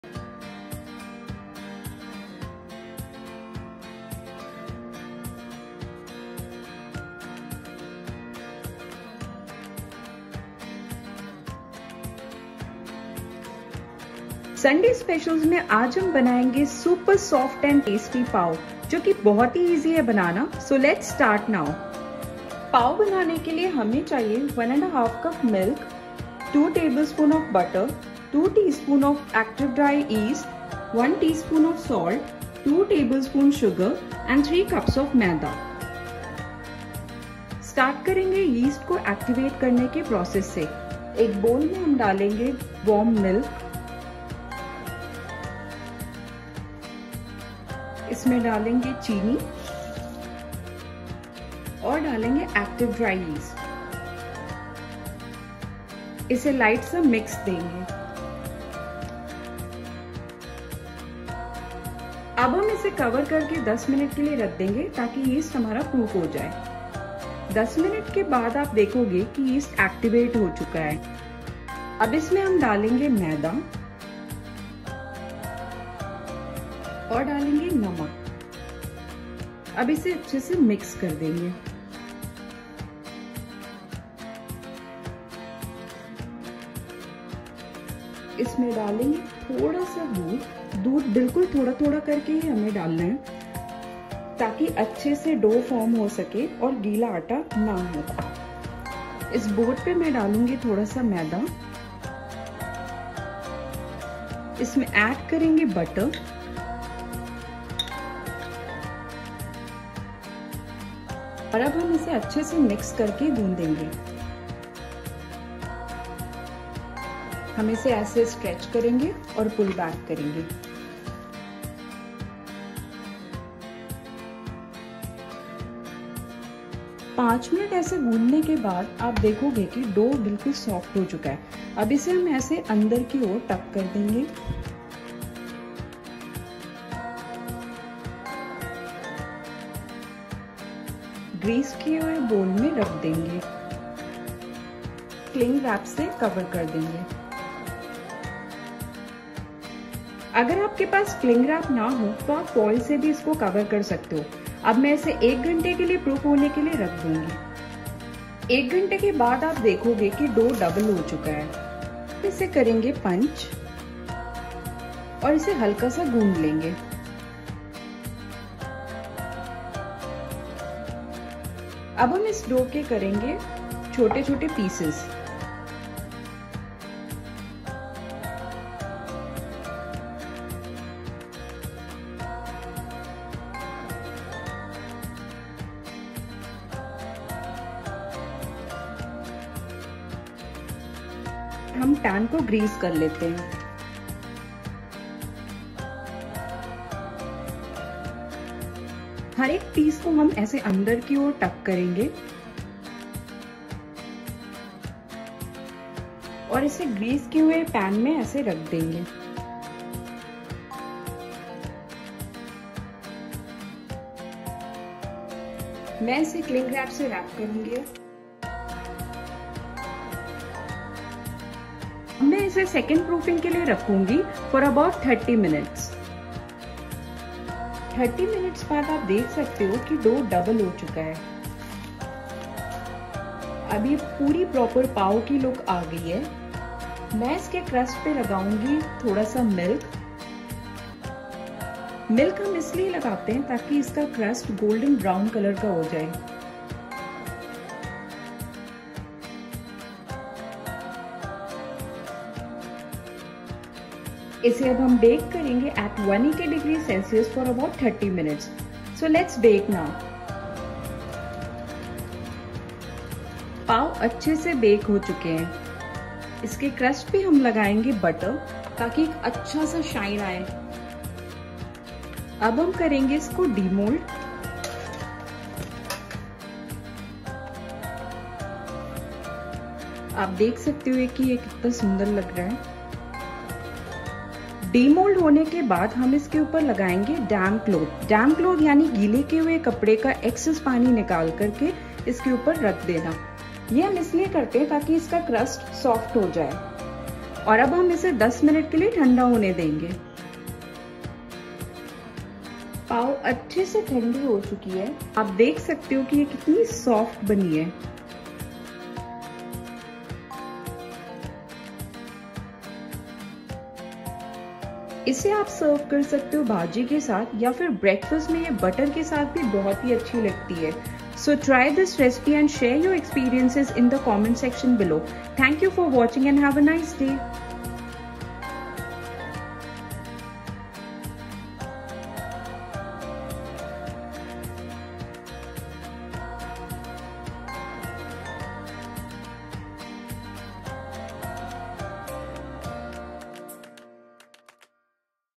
Pav Recipe | Eggless Pav Bread | Ladi Pav | Super Soft Pav Recipe | Homemade Eggless Pav Bread
Homemade Pav Recipe with all the tips & tricks to make a Perfect Soft Pav-

[Ingredients]

For yeast activation-
1 cup warm milk
2 tbsp sugar
2 tsp yeast

For Dough-
3 cup Maida
1 tsp salt
2 tbsb butter
Some milk to make dough (add as required)

1st Proofing for 1 hour
2nd Proofing for 30 mins

Preheat @180 c for 10 mins & then Bake at 180 c for about 25-30 minutes